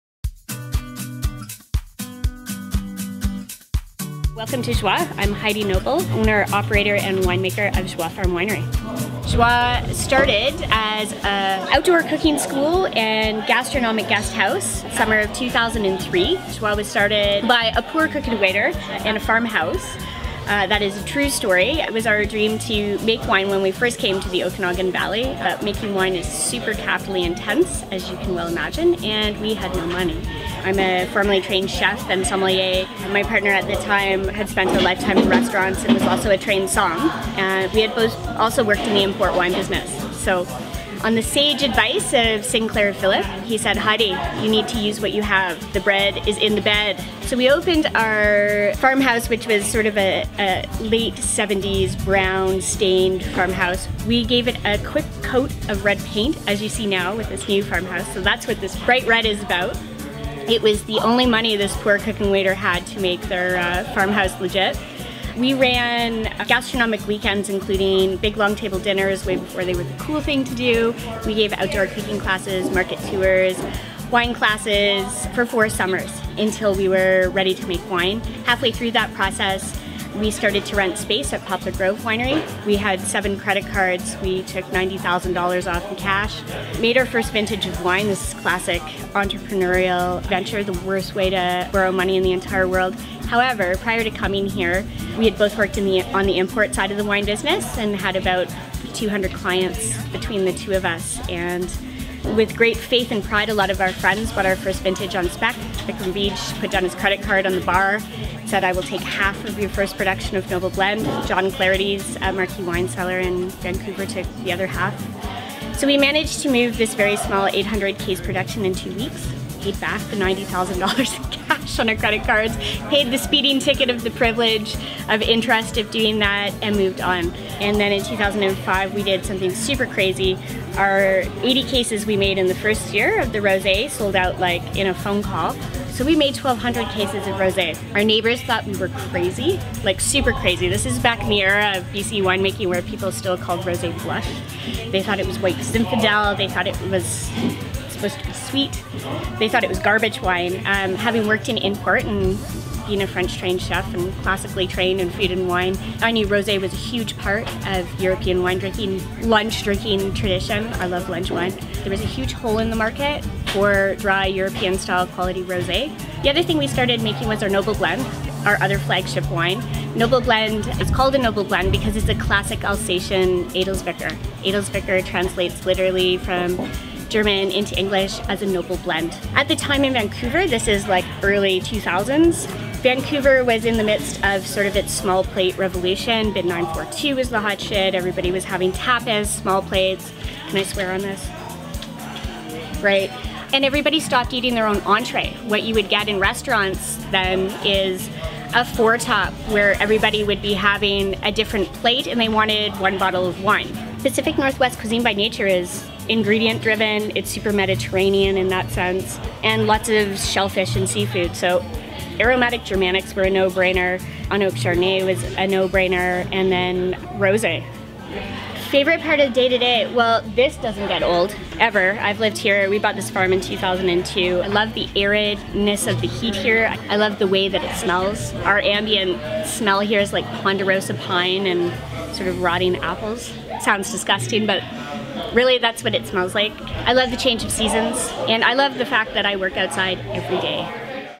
Heidi Noble: On Starting A Winery
Hanging out with Heidi Noble at the lovely JoieFarm Winery talking about risking it all to make a vision happen.

Words that resonate with us more than you think!

Find us elsewhere online!